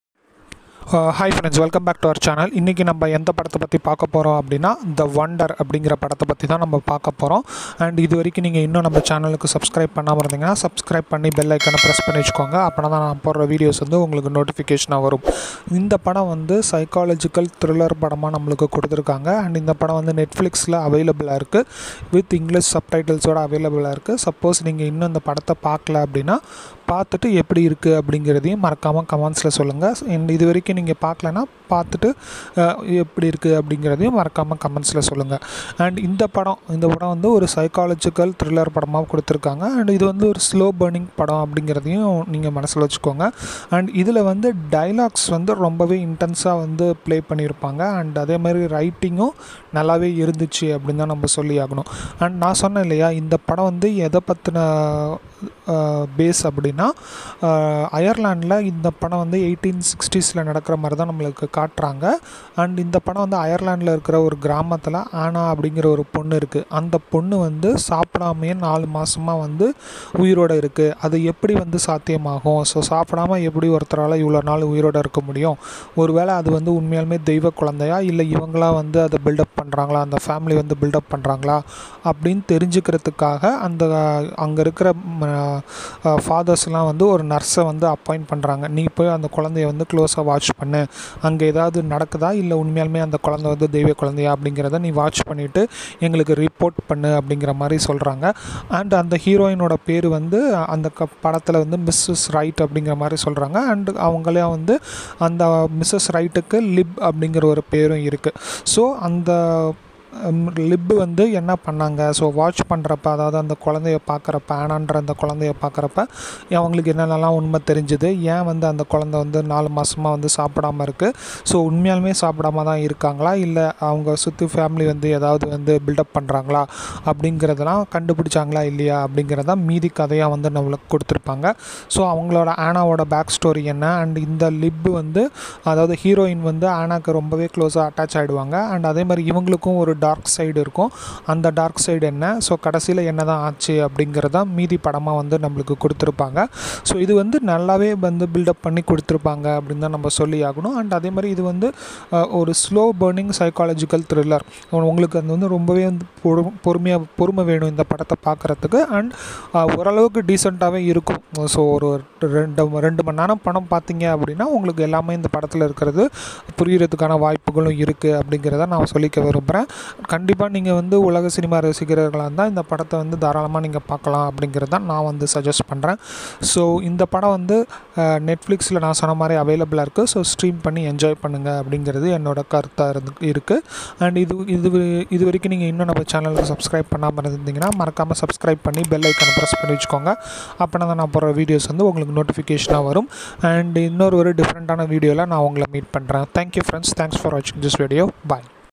The wonder movie review in Tamil justmyreviews thewondermoviereview
In this video you will be watching my Review on recently released movie in Netflix which is psycholgical thriller movie. Please note this is only based on my opinion this may vary from person to person justmyreviews thewonder thewondermoviereview